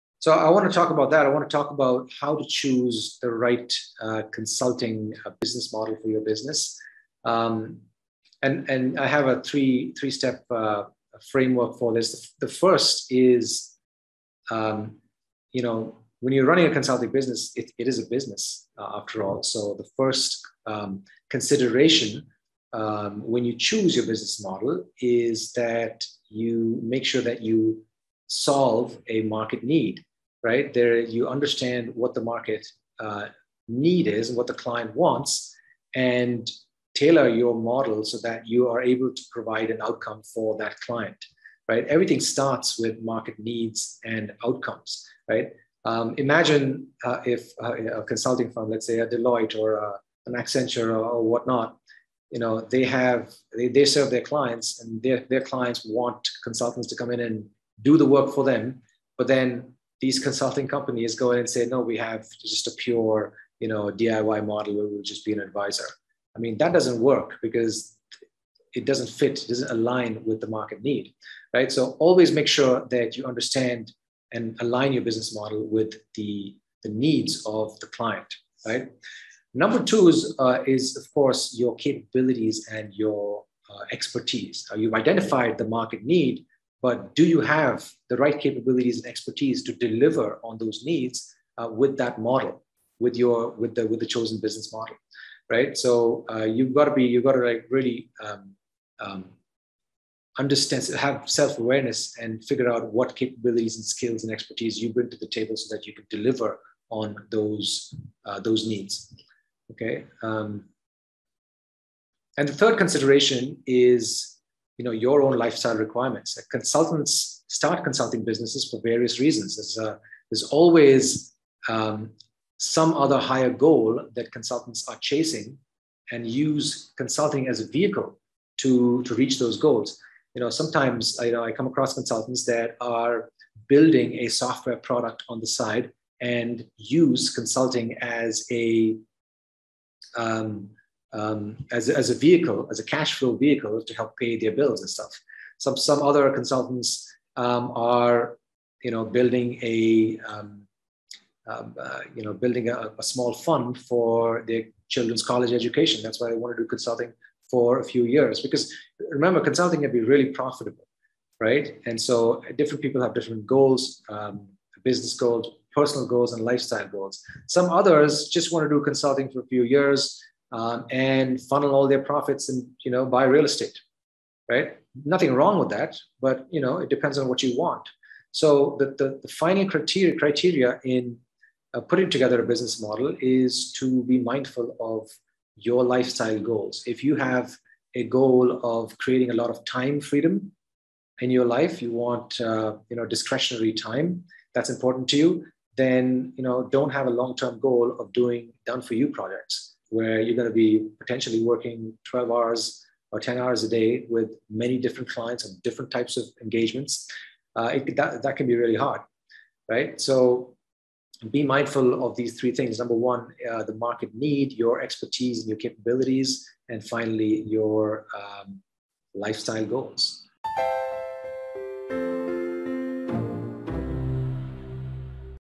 How to Pick a Business Model for Your Consulting Business | Best Consulting Business Model :
Consulting business owners who rely on their technical expertise to deliver business growth leaves many opportunities on the table.

If you're simply too busy delivering client projects to pay attention to your sales and marketing functions, then you'll eventually struggle.

But even before you dive into sales and marketing, you want to make sure your business model is set up properly for success.

To me, a business model is simply how your business is structured so it delivers value to your clients and to you.

Here's a snapshot of the various types of business models that work in consulting:

✅ 1. Done for you (DFY): You do the work for your clients. Most consulting companies are DFY-based. They get you in, you deliver.

✅ 2. Done with you (DWY): Part of the work is done by the client and part by you.

✅ 3. DIY (Pure advisory and/or Executive coaching): You offer intellectual property and advice alone and your clients implement.

✅ 4. DIY (Training): Skills transfer from consultant to client and/or teams.

✅ 5. Hybrid: Any of these models mixed and matched to deliver results.

But how do you decide which model (or combination of models) is right for you?